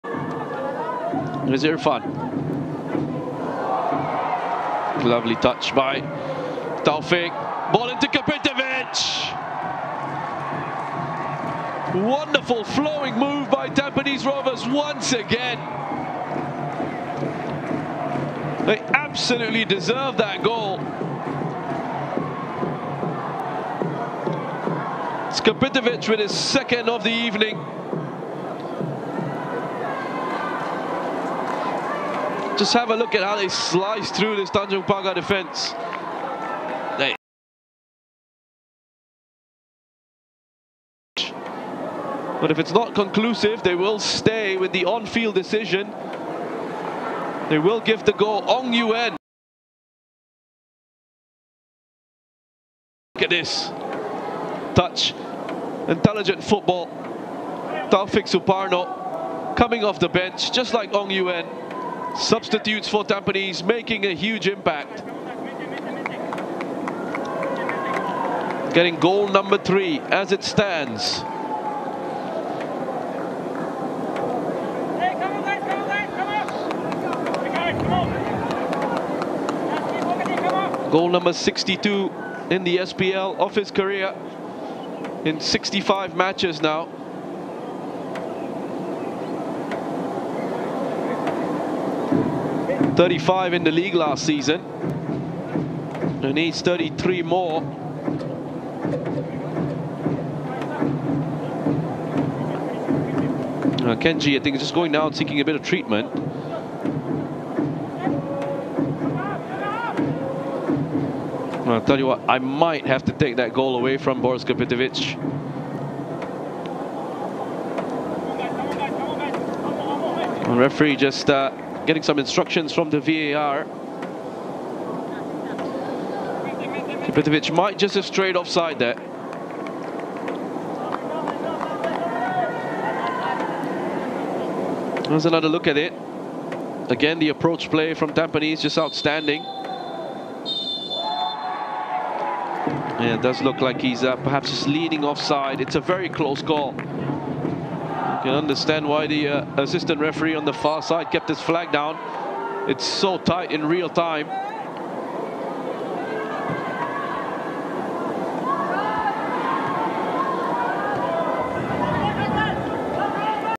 Tampines' Kopitovic ruled JUST offside vs Tanjong Pagar as goal is disallowed | SPL 2023 Moments
It was a real tight angle which could have resulted in Boris Kopitovic grabbing his second goal against the Jaguars but ultimately things did not go his or the Stags' way.